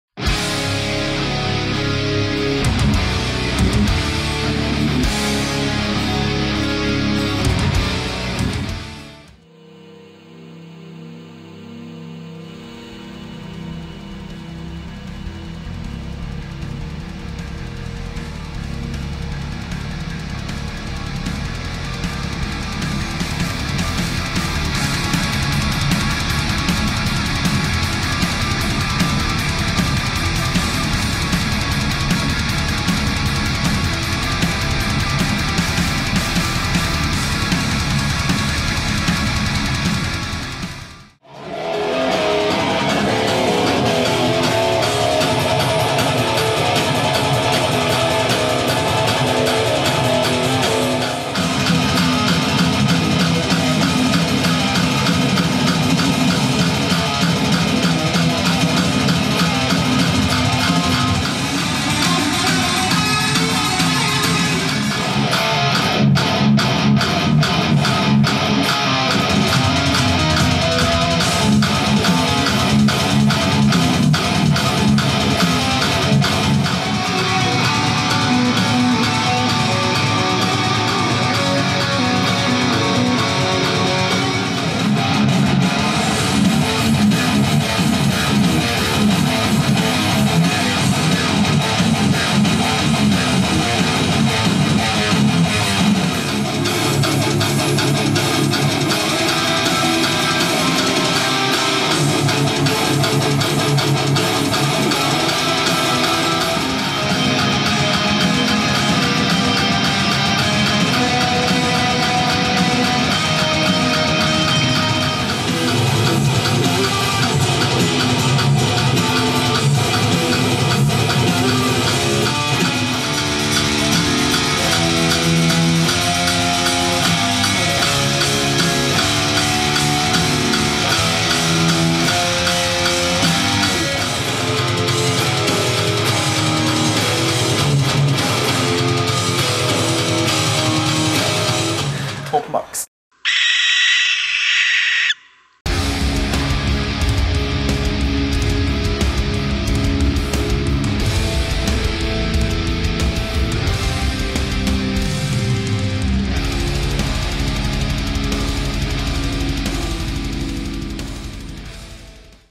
THE WRETCHED END guitar session 2011
The Wretched End guitar session 2011 with Samoth and Cosmo.

WATCH OUT FOR NEW ALBUM 2012!